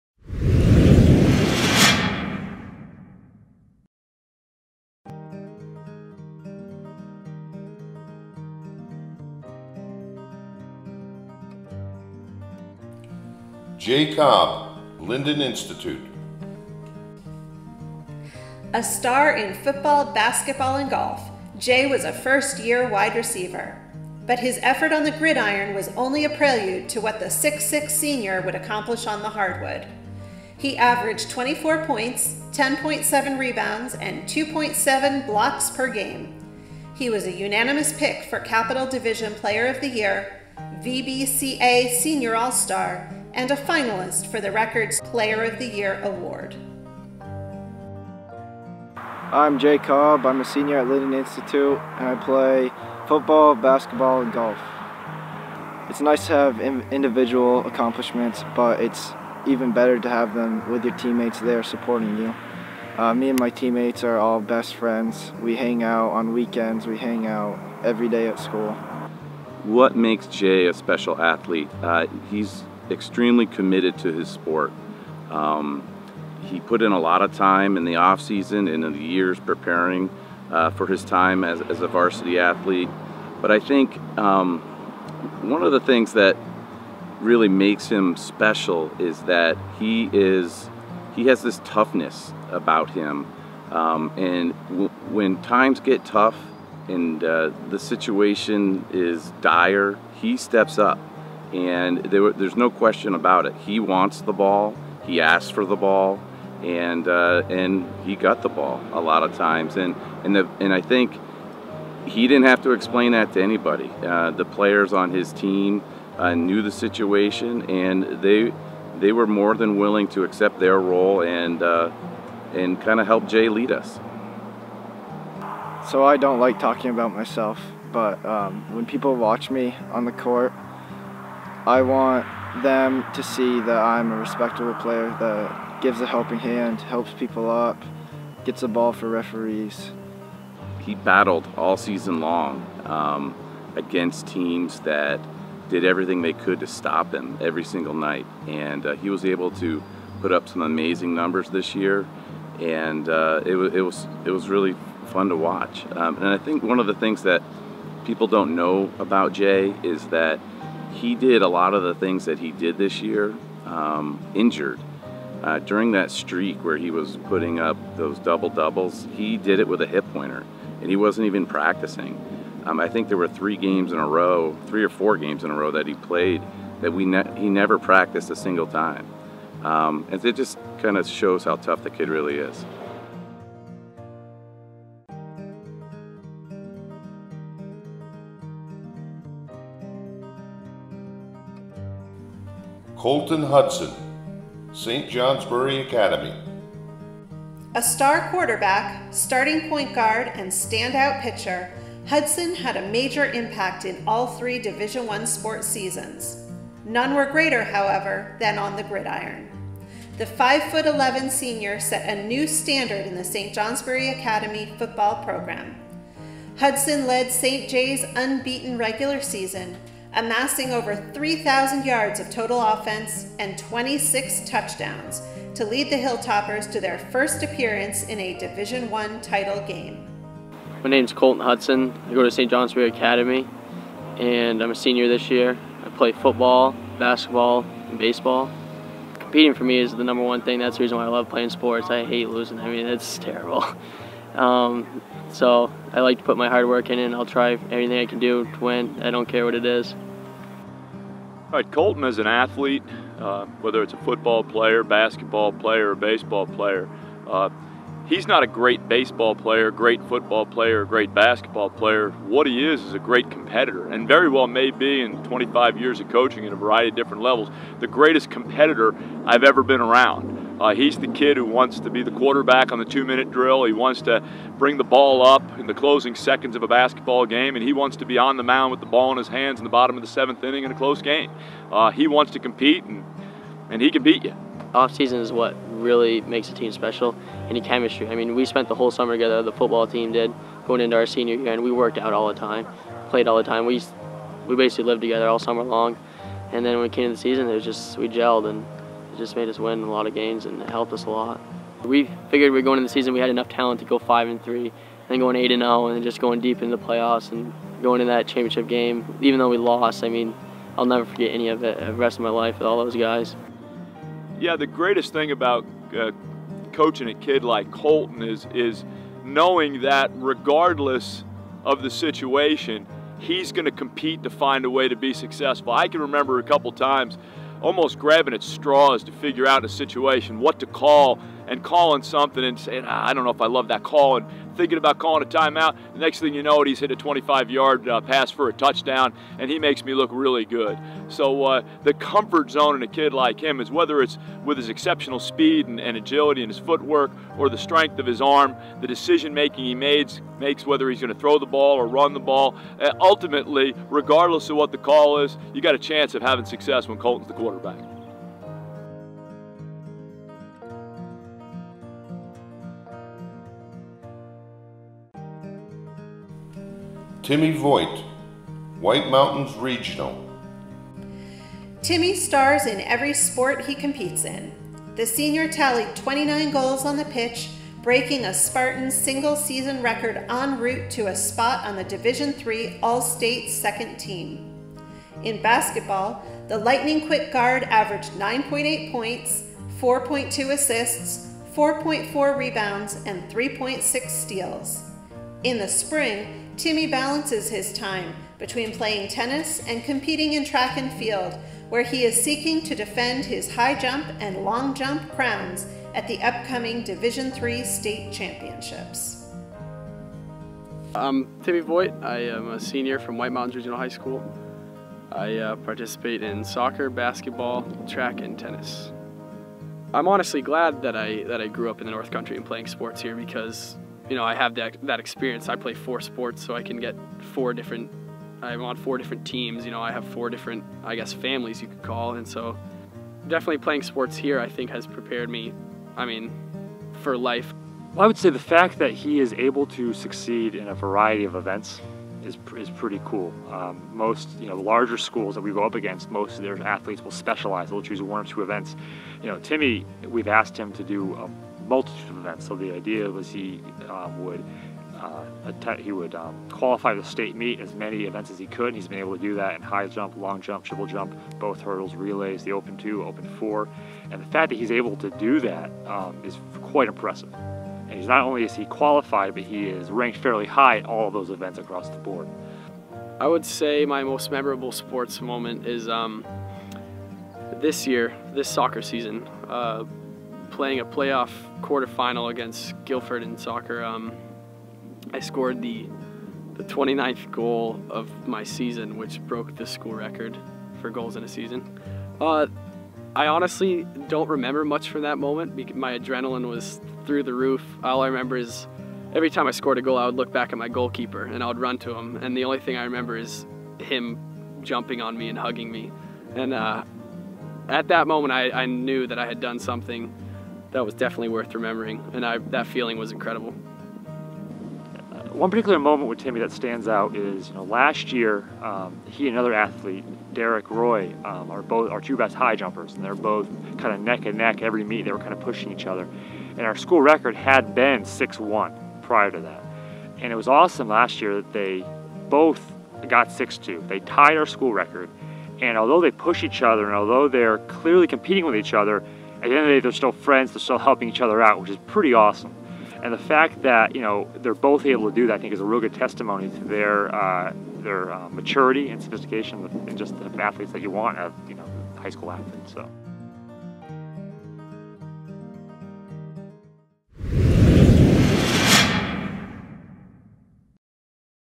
NNEKY Awards: 2014-15 Male Athlete of the Year Finalists
The Caledonian Record presents the 2014-15 NNEKY Male Athlete of the Year Finalists. WMR's Timmy Voigt won the prize.